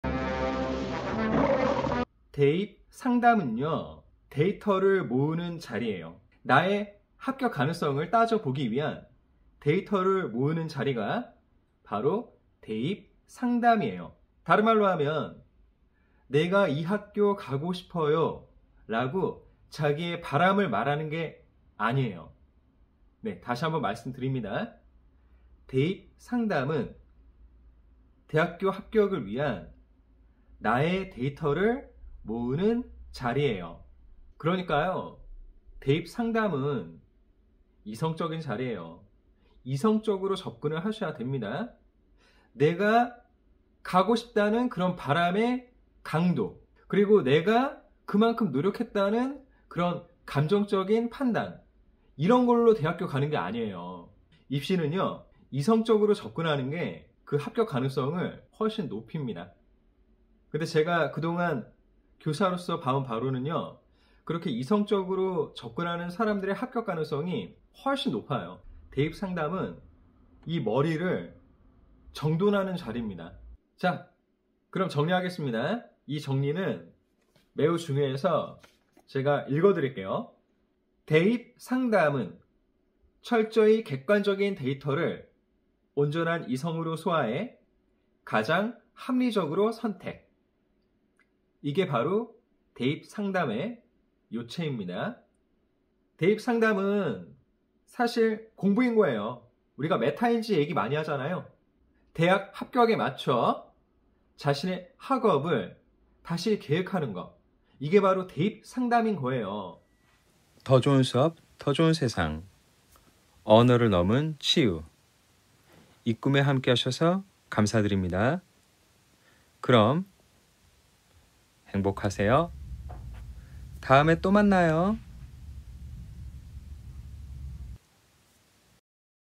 대입 상담 활용법 2분 요약!
합격 가능성 높이는 대입 상담법을
2분으로 요약했어요!

풀영상은 아래로 가시면 되요.
이 내용은 포스트로도 게시했습니다.

수험생 여러분, 그리고 학부모님들,
응원합니다~